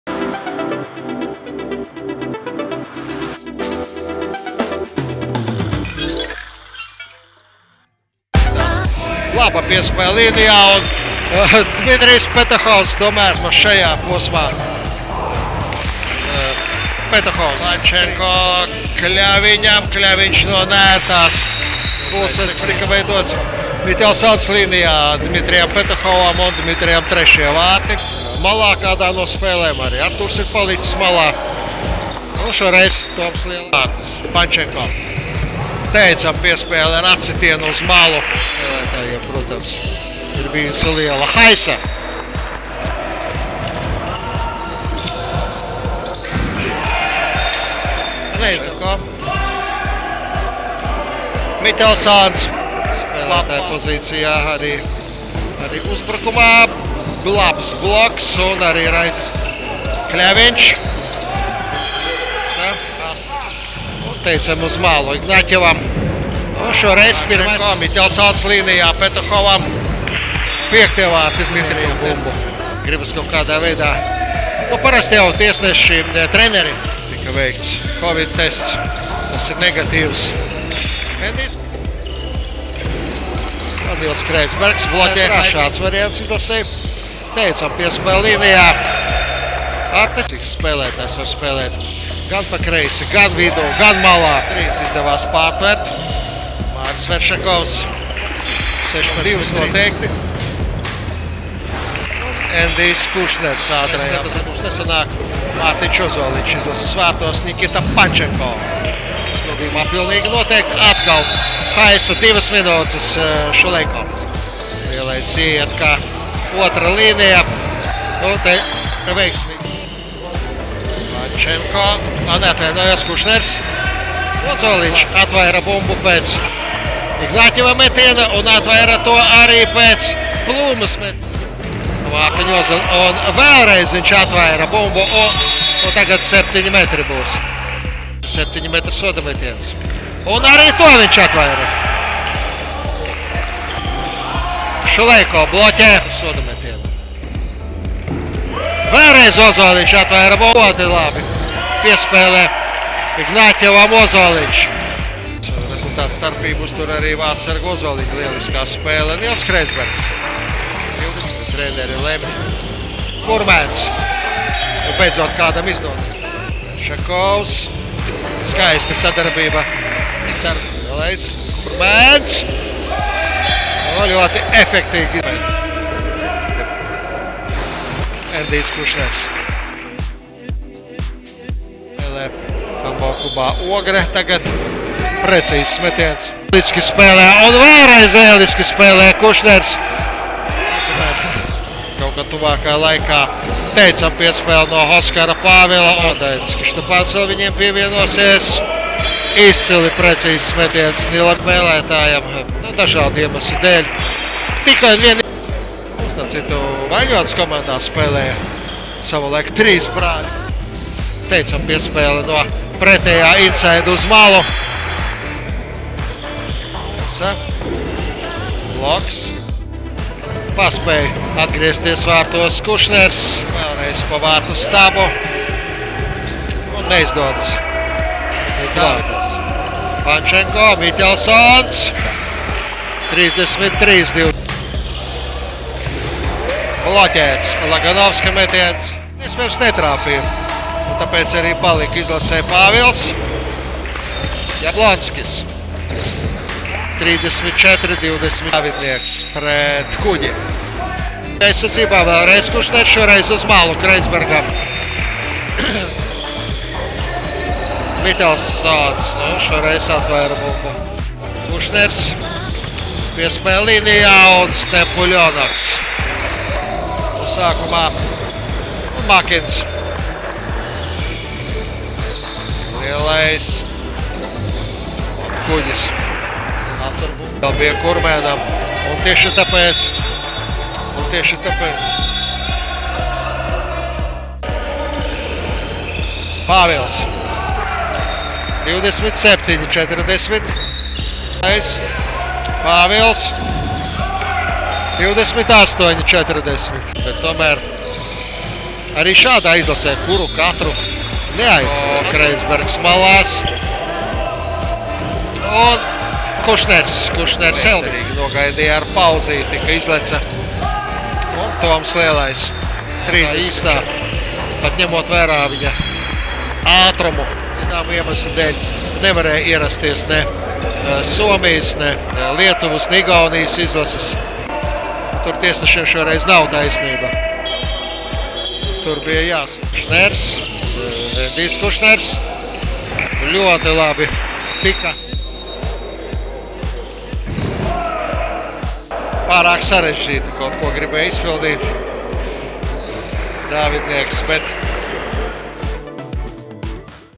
Spēles momenti: Latvija - SynotTip klubu izlase | 3. janvārīs
Gatavojoties Eiropas čempionāta kvalifikācijas spēlēm Latvijas handbola izlases spēlētāji šajā nedēļas nogalē aizvadīja divas treniņspēles pret SynotTip handbola virslīgas klubu izlasi, kuras sastāvā spēlēja Latvijas B izlases spēlētāji kopā ar SynotTip handbola virslīgā spēlējošiem leģionāriem.

Arī otrajā spēlē nekādu pārsteigumu nebija un Latvijas izlase uzvarēja ar pārliecinošu rezultātu 45:31.